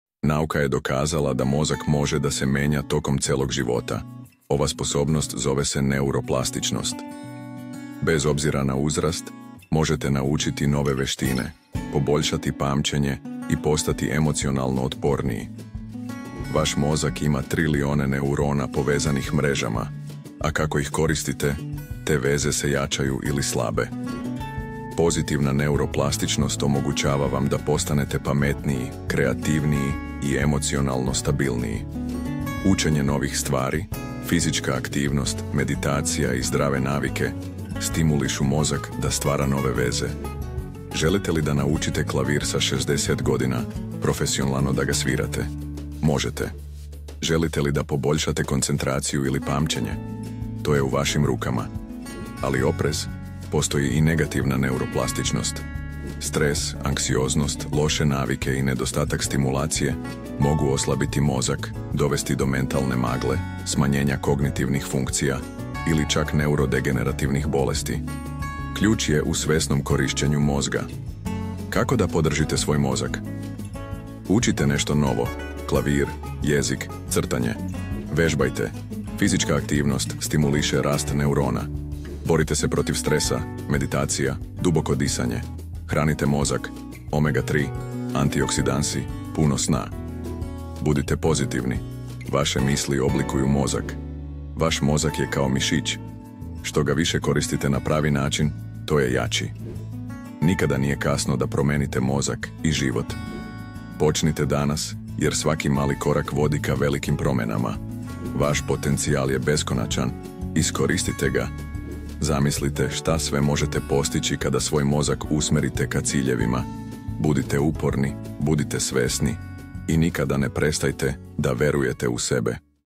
OTKRIJTE POTENCIJAL SVOG MOZGA: NAUKA O NEUROPLASTIČNOSTI I DOŽIVOTNOM RASTU
Pogledajte Opis: Kako možete promeniti svoj mozak u bilo kom dobu: Snaga neuroplastičnosti
Mnogi veruju da nakon određenog uzrasta, posebno nakon 25. godine života, mozak više nije sposoban za promene. Međutim, nauka je dokazala da je ovo u potpunosti netačno. Vaš mozak ima neverovatnu sposobnost da se menja i prilagođava tokom celog života, čak i u kasnijim godinama. Ova sposobnost poznata je kao neuroplastičnost, i ona je ključna za razumevanje kako možemo da unapredimo svoje kognitivne funkcije, veštine i emocionalno zdravlje.

Šta je neuroplastičnost?
Neuroplastičnost je sposobnost mozga da reorganizuje svoje strukture, funkcije i veze između neurona (moždanih ćelija) kao odgovor na iskustva, učenje, vežbanje i čak i misli. To znači da vaš mozak nije fiksiran u jednom stanju – on je dinamičan i može se prilagođavati tokom celog života.
U vašem mozgu postoji trilijon moždanih ćelija, a svaka od njih je povezana sa hiljadama drugih. Te veze se nazivaju sinapse, i one su osnova za sve što radimo, mislimo i osećamo. Kako koristite svoj mozak, te veze se jačaju ili slabe. Ako stalno koristite određene veze (na primer, učenjem nove veštine), one će postati jače. Ako ih zanemarujete, one će slabiti.

Kako možete promeniti svoj mozak?
Evo kako možete iskoristiti snagu neuroplastičnosti:
Učenje novih veština u bilo kom dobu
Želite li da naučite da svirate klavir sa 60 godina? Možete. Želite li da naučite novi jezik, da postanete bolji u matematici ili da savladate bilo koju drugu veštinu? To je moguće zahvaljujući neuroplastičnosti. Kada učite nešto novo, vaš mozak stvara nove veze između neurona. Što više vežbate, te veze postaju jače, a vaša veština postaje sve bolja.

Pozitivna neuroplastičnost
Pozitivna neuroplastičnost odnosi se na promene u mozgu koje vode ka poboljšanju kognitivnih funkcija, pamćenja, koncentracije i emocionalnog zdravlja. Na primer:
Učenje i vežbanje: Čitanje knjiga, rešavanje zagonetki, učenje novih stvari.
Fizička aktivnost: Vežbanje poboljšava protok krvi u mozgu i stimuliše rast novih neurona.
Meditacija i mindfulness: Ove prakse mogu smanjiti stres i poboljšati koncentraciju.
Zdrav način života: Pravilna ishrana, dovoljno sna i izbegavanje štetnih navika (kao što je pušenje) podržavaju zdravlje mozga.

Negativna neuroplastičnost
Nažalost, mozak može da se menja i na negativan način. Ako se izlažete stresu, negativnim mislima, lošim navikama ili nedostatku stimulacije, vaš mozak može da oslabi. Na primer:
Stres i anksioznost: Hronični stres može dovesti do smanjenja volumena mozga, posebno u oblasti koja upravlja pamćenjem i emocionalnom regulacijom.
Loše navike: Preterano korišćenje tehnologije, nedostatak sna, nezdrava ishrana i nedostatak fizičke aktivnosti mogu dovesti do degeneracije mozga.
Mentální magla: Ako ne koristite svoj mozak na pravi način, možete osetiti smanjenje kognitivnih sposobnosti, kao što su gubitak koncentracije, pamćenja i jasnoće mišljenja.

Kako zaustaviti negativne promene i podržati zdravlje mozga?
Borite se protiv stresa
Stres je jedan od najvećih neprijatelja mozga. Prakticirajte tehnike opuštanja, poput meditacije, dubokog disanja ili joge. Ove aktivnosti mogu smanjiti nivo kortizola (hormona stresa) i podržati neuroplastičnost.

Ostanite aktivni
Fizička aktivnost ne samo da je dobra za vaše telo, već i za vaš mozak. Vežbanje povećava protok krvi u mozgu i stimuliše rast novih neurona.

Hranite svoj mozak
Ishrana bogata antioksidansima, omega-3 masnim kiselinama i vitaminima može podržati zdravlje mozga. Konzumirajte hranu kao što su orasi, riba, borovnica, zeleno povrće i tamna čokolada.
Učite nešto novo svakog dana
Čak i male promene u vašoj rutini mogu stimulisati mozak. Probajte nešto što nikada pre niste radili – naučite novi jezik, igrajte šah, crtajte ili svirajte instrument.
Budite svesni svojih misli
Vaše misli imaju moć da oblikuju vaš mozak. Ako stalno mislite negativno, to može dovesti do negativne neuroplastičnosti. Usmerite svoje misli ka pozitivnim i konstruktivnim stvarima.

Zaključak
Vaš mozak je neverovatno moćan organ koji ima sposobnost da se menja i prilagođava tokom celog života. Bez obzira na vaš uzrast, nikada nije kasno da počnete da brinete o svom mozgu i da ga stimulišete na pravi način. Korišćenjem snage neuroplastičnosti možete postati pametniji, zdraviji i srećniji.
Zapamtite: Vaš mozak je kao mišić – što ga više koristite na pravi način, to će biti jači. Nemojte dozvoliti da vas uzrast ili negativne navike spreče da ostvarite svoj pun potencijal. Počnite danas i posvetite se sebi i svom mozgu – jer on zaslužuje vašu pažnju i brigu.